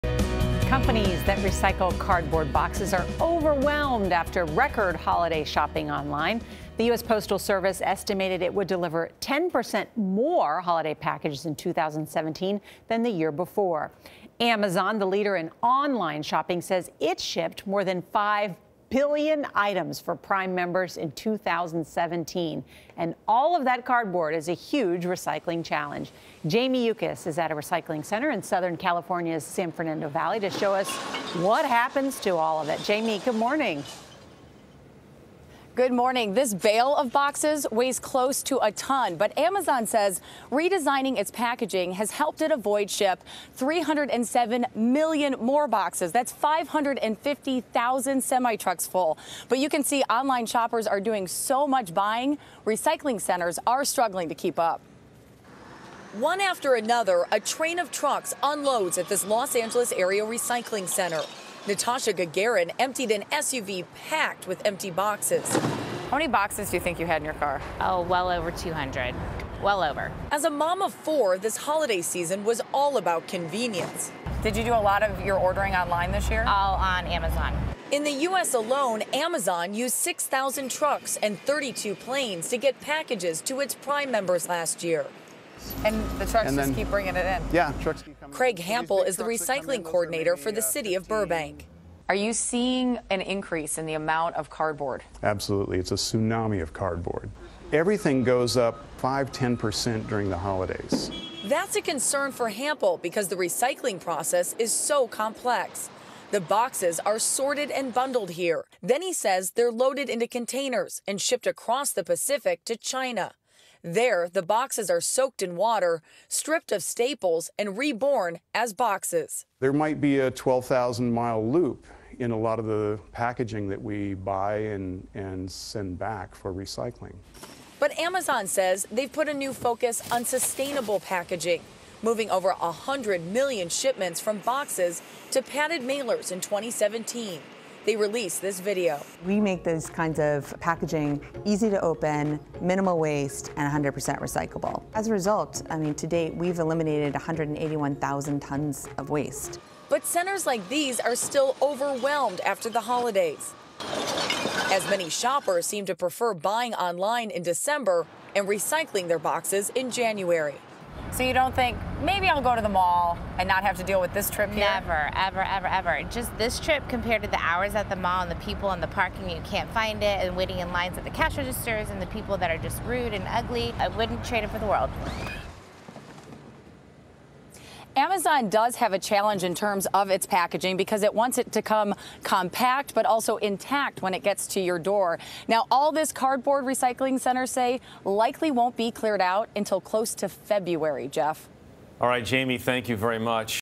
Recycle centers overwhelmed with boxes post-holiday shopping
Companies that recycle cardboard boxes are overwhelmed in the wake of record holiday shopping online. Amazon, the leader in online shopping, said it shipped more than five billion items for Prime members in 2017. Now they're putting a new focus on sustainable packaging. Jamie Yuccas reports.